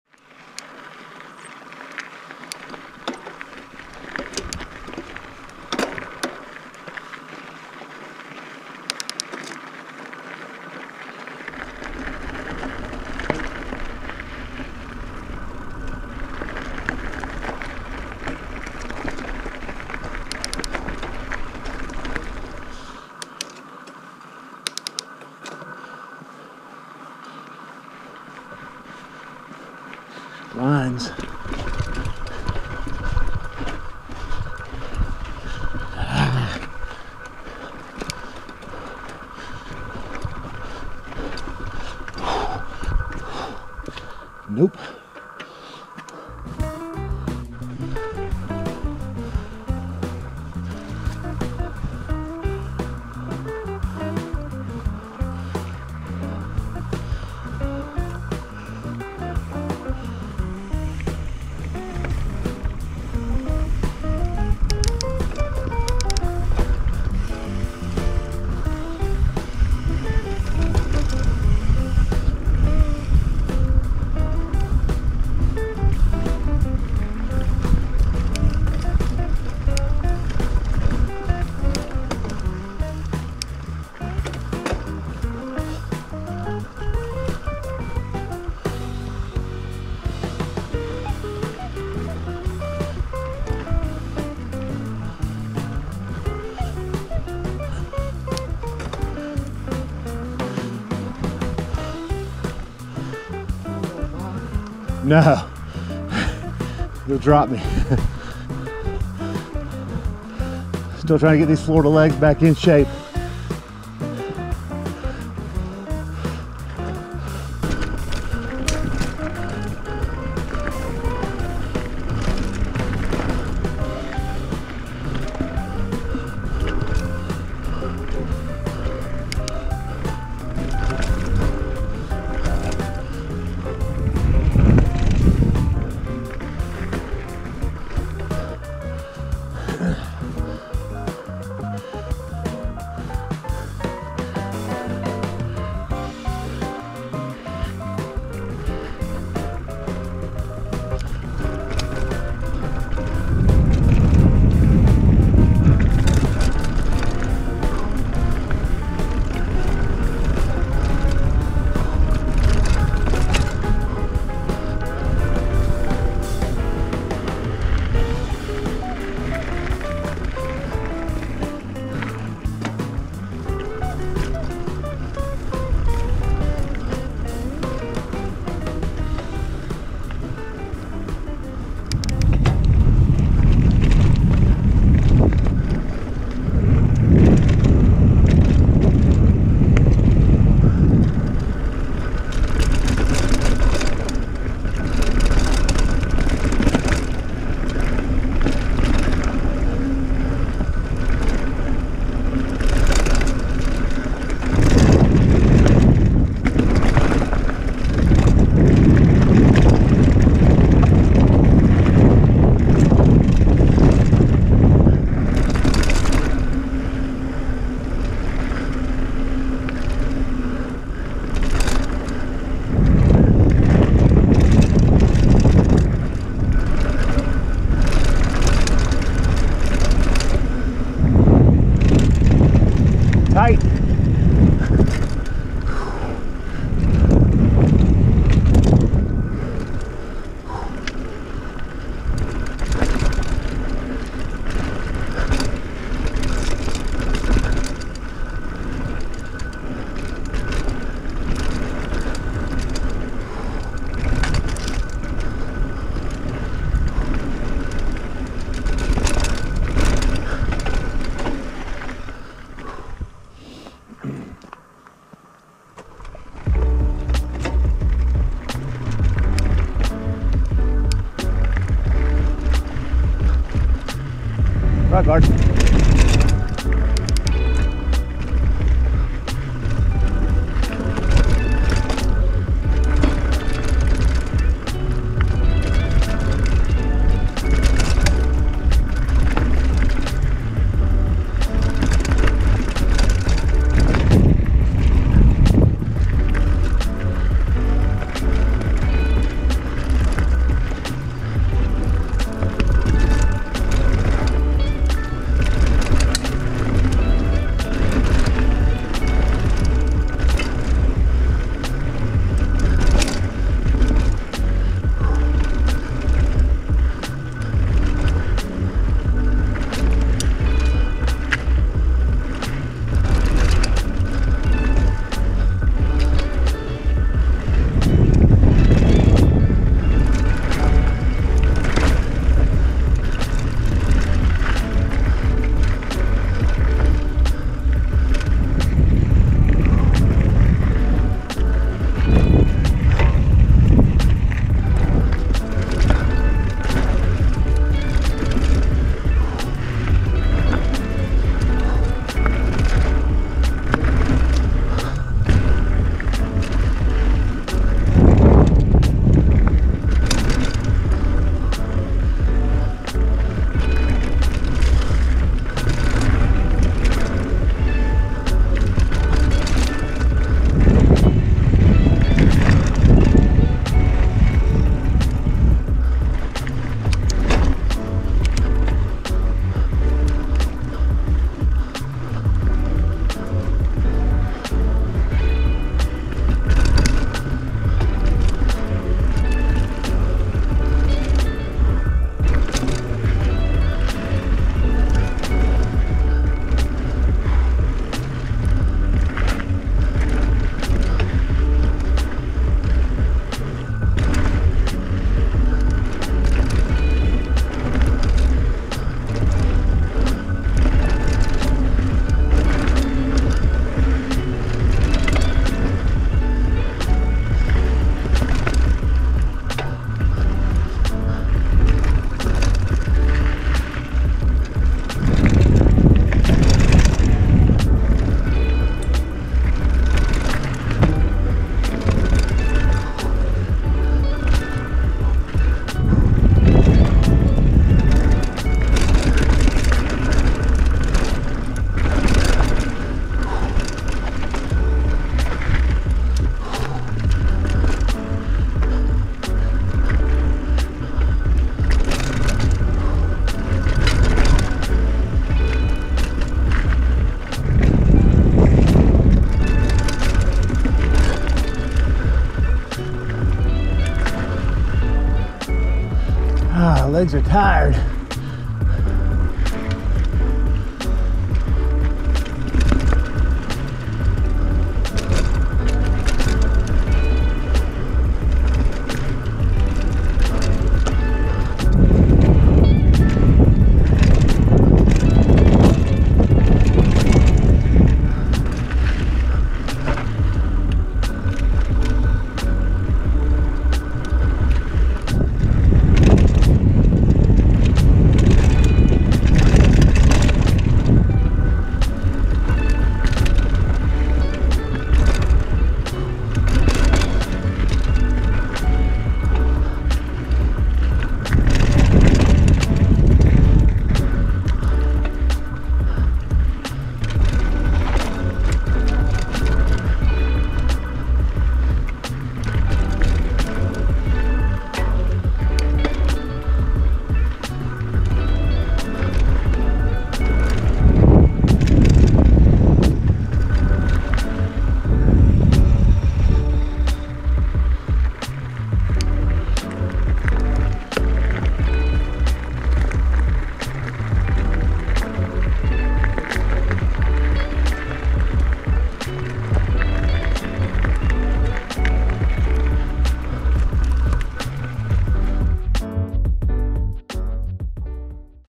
Ridgeline - Dupont State Forest
I finally get out to ride a trail I've been wanting to ride for quite a while - Ridgeline.  This trail is located just outside Brevard, NC in the Dupont State Forest and is famous for its flow and berms.

The climb up isn't fun, but the trip down makes it all worth it.  This is a legendary trail on the East Coast, so if you're in the area you should definitely make plans to fit in a ride.